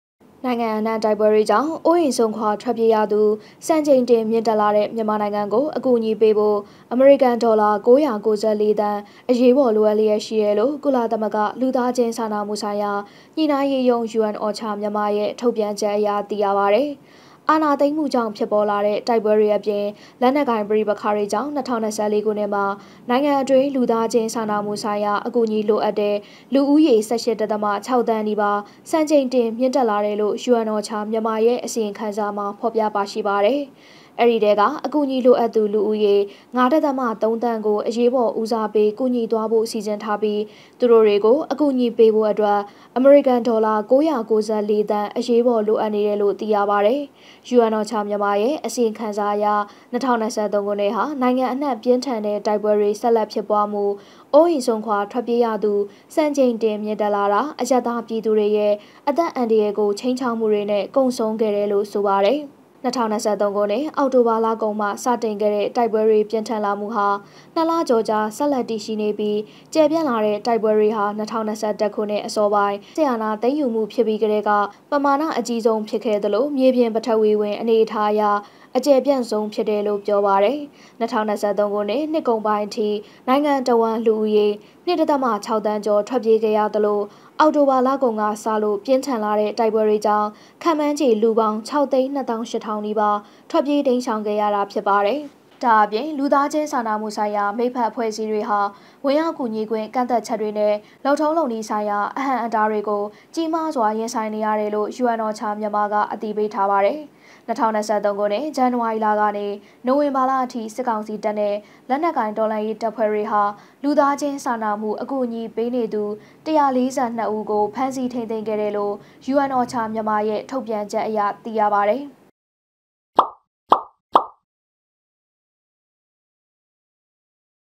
TLT Tv||- မြန်မာကို ကူညီပေးဖို့ ဒေါ်လာသန်း ၉၀၀ ကျော် အရေးပေါ်လိုအပ်နေတယ်လို့ ကုလ ပြော
နိုင်ငံအနှံ့တိုက်ပွဲတွေကြောင့် အိုးအိမ်စွန့်ခွာထွက်ပြေးရသူ စံချိန်တင်မြင့်တက်လာတဲ့ မြန်မာနိုင်ငံကို ကူညီပေးဖို့ အမေရိကန်ဒေါ်လာ ၉၉၄ သန်း အရေးပေါ်လိုအပ်လျက်ရှိတယ်လို့ ကုလသမဂ္ဂ လူသားချင်းစာနာမှုဆိုင်ရာ ညှိနှိုင်းရေးရုံး UNOCHA Myanmar ရဲ့ ထုတ်ပြန်ချက်အရ သိရပါတယ်။